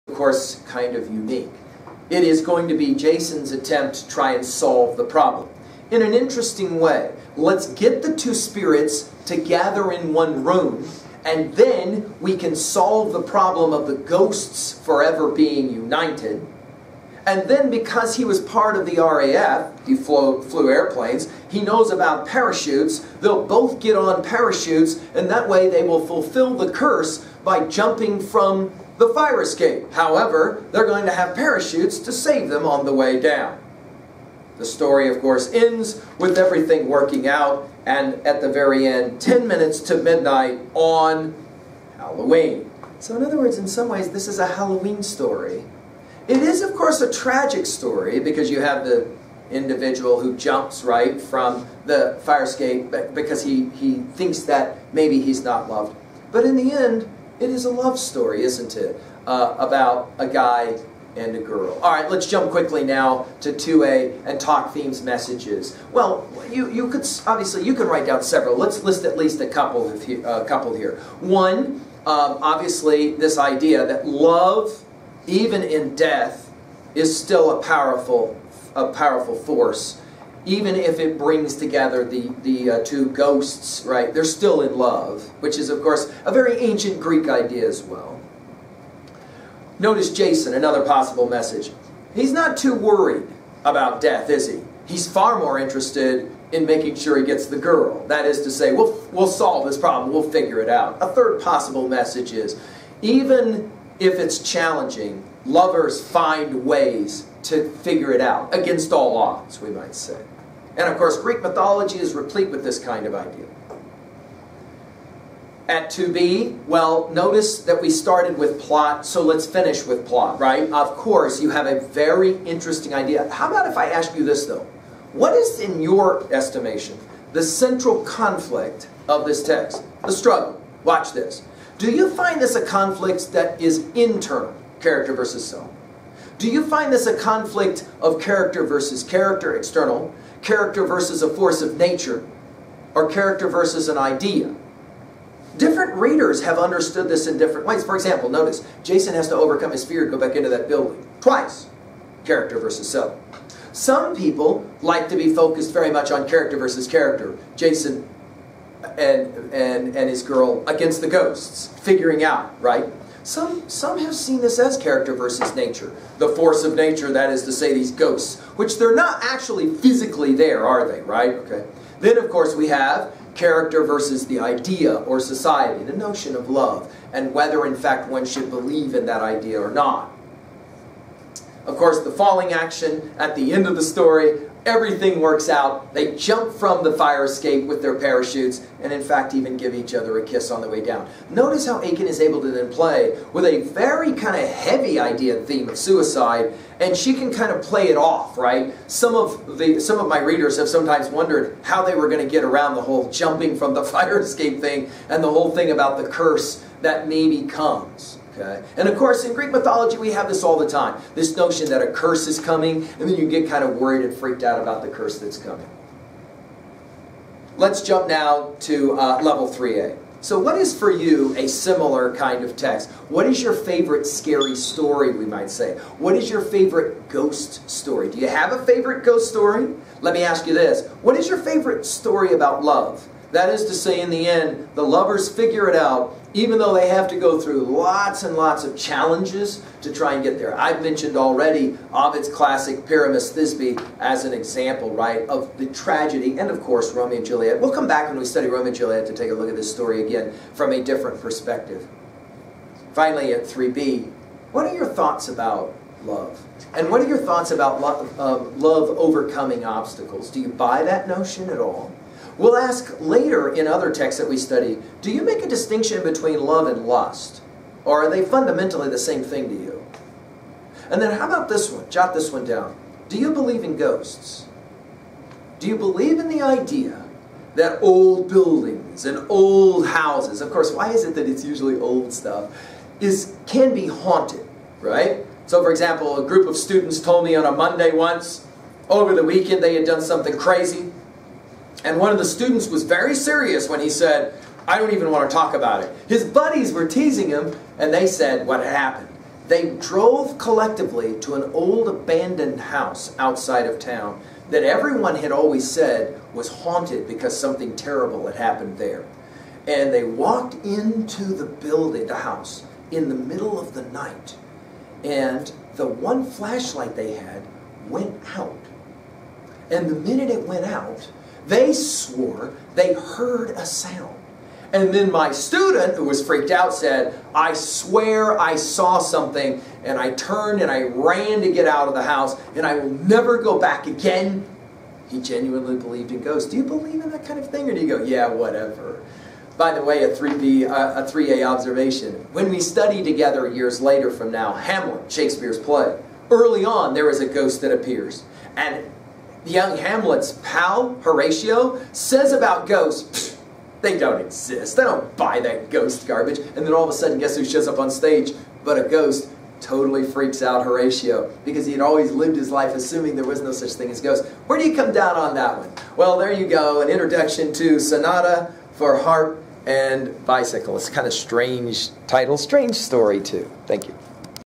Aiken’s Sonata for Harp and Bicycle, C
McGee of 303 and Learnstrong.net lectures on Aiken’s Sonata for Harp and Bicycle, C